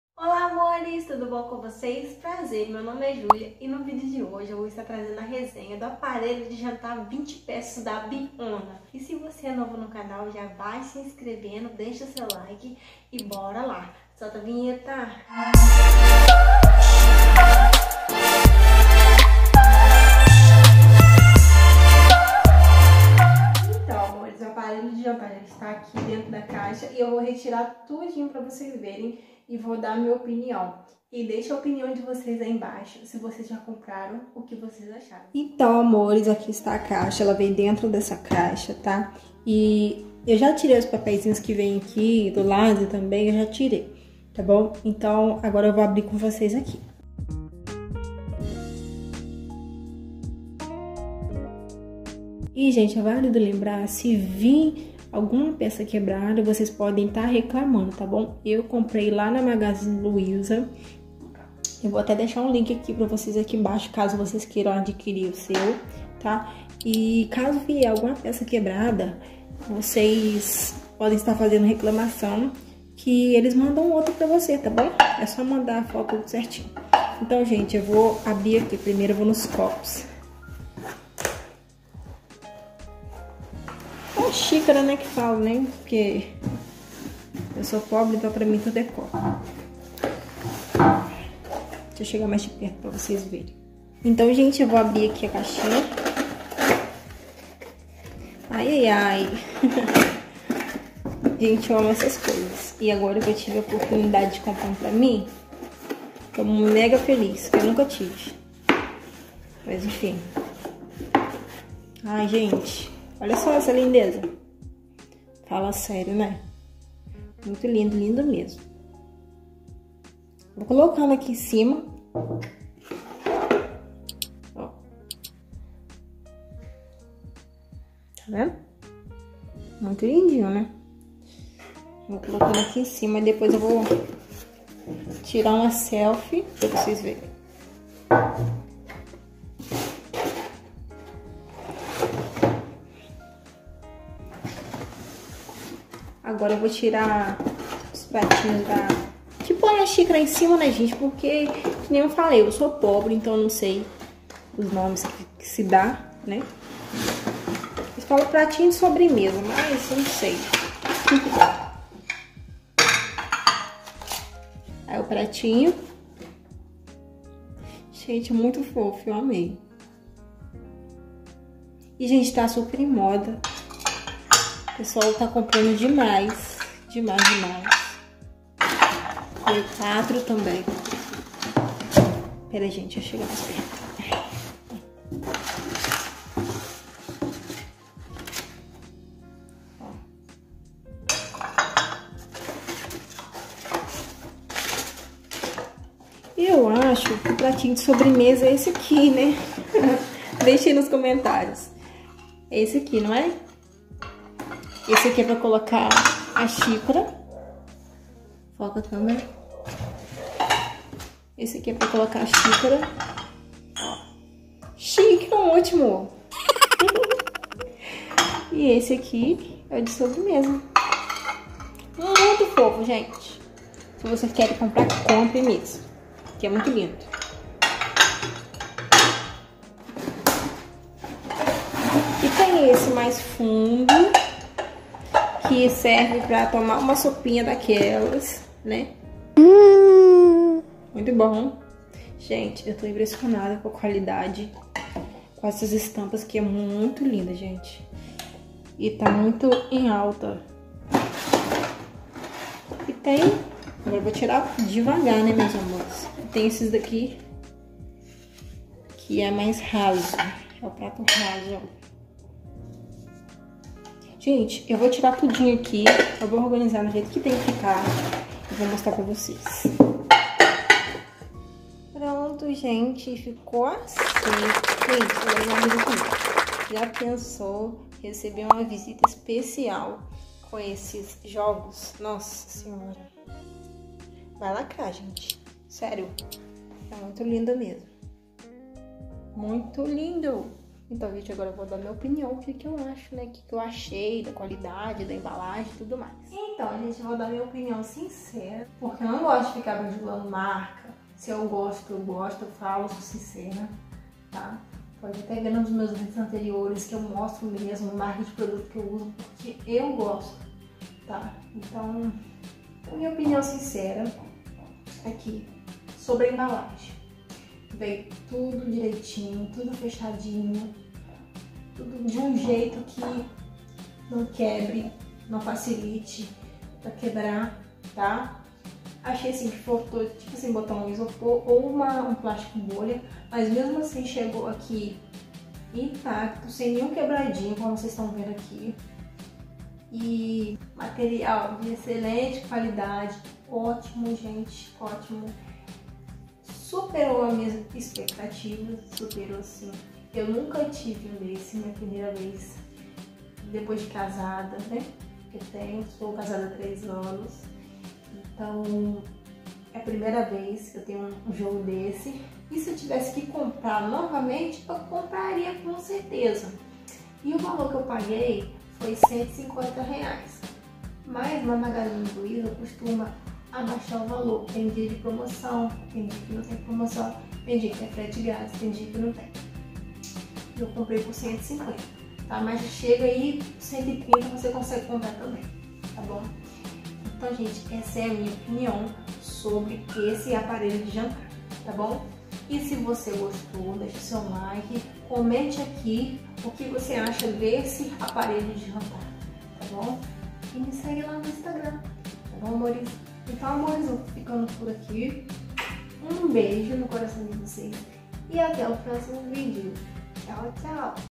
RESENHA APARELHO DE JANTAR 20 PEÇAS BIONA
Oiii amores espero que vocês gostem do vídeo.. no vídeo eu mostro como é o aparelho de jantar e dou a minha opinião sincera sobre ele

Caso você tenha interesse em comprar e só apertar no link abaixo e super seguro tudo feito pelo site da magazine Luiza